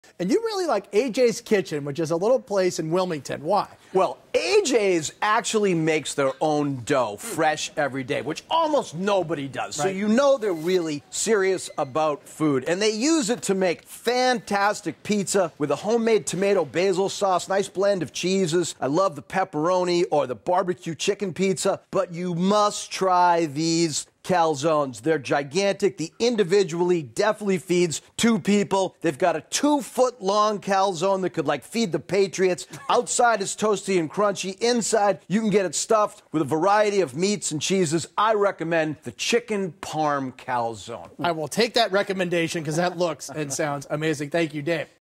AJ's Kitchen - Wilmington MA
"AJ's actually makes their own dough, fresh every day, which almost nobody does, so you know they're really serious about food. And they use to make fantastic pizza with a homemade tomato basil sauce, a nice blend of cheeses. I love the pepperoni or the barbecue chicken pizza. But you must try these calzones. They're gigantic. The "Individual" definitely feeds two people. They've got a 2 foot long calzone that could feed the Patriots. Outside is toasty and crunchy, inside you get it stuffed with a variety of meats and cheeses. I recommend the Chicken Parm Calzone."